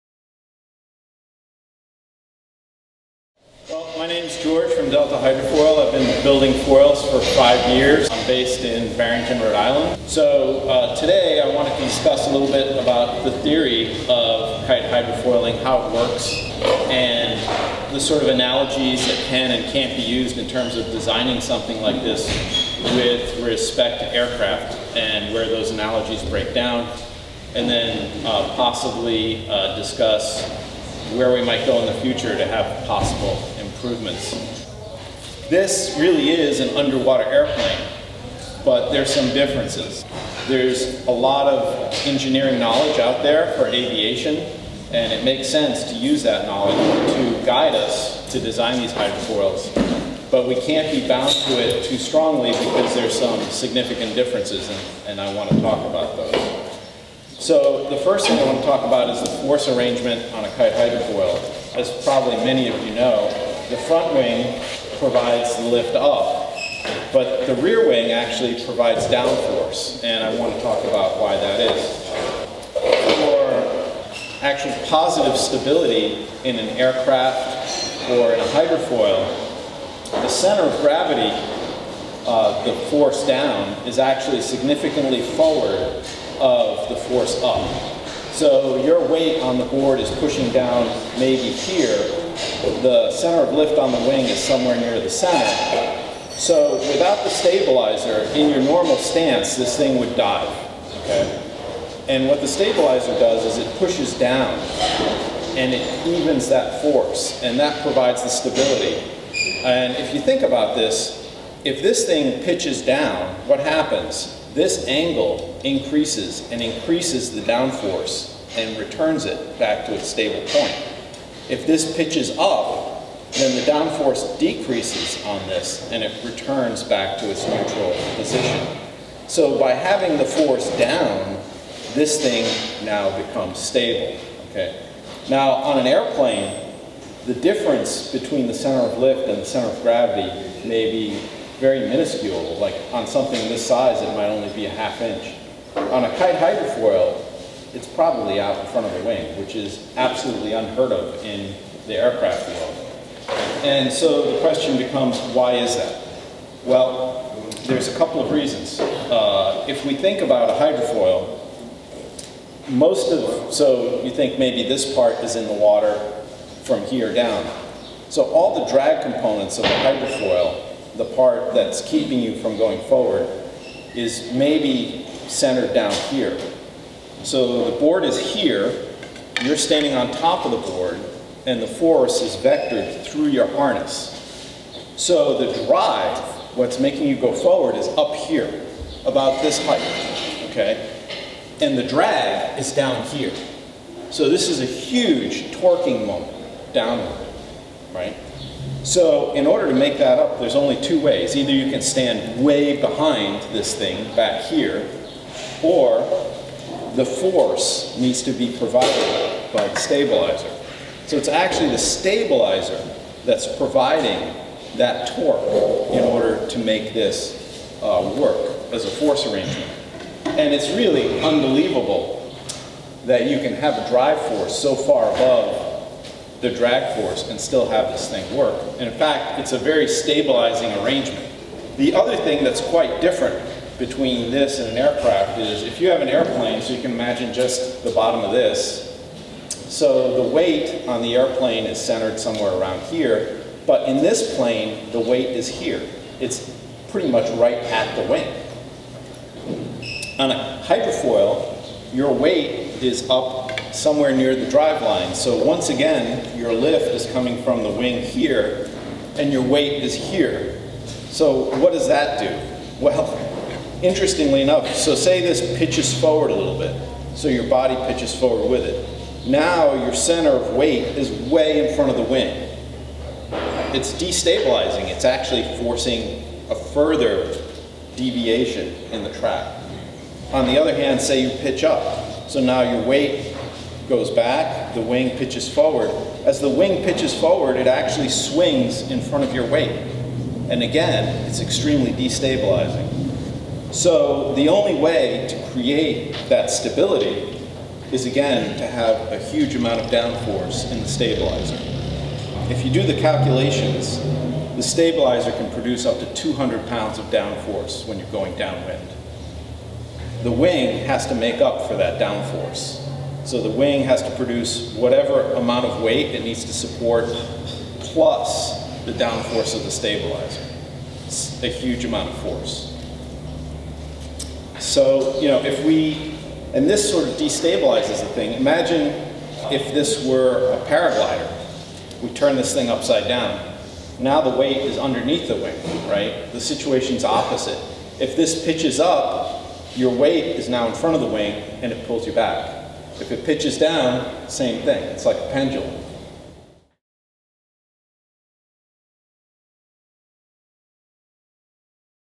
Kite Hydrofoil Lecture: Part 1 - Theory
Dr George from Delta Hydrofoil talks about theory behind kite hydrofoils. This is Part 1 of the lecture at the Hydrofoil Pro Tour in La Ventana, MX (March 2017)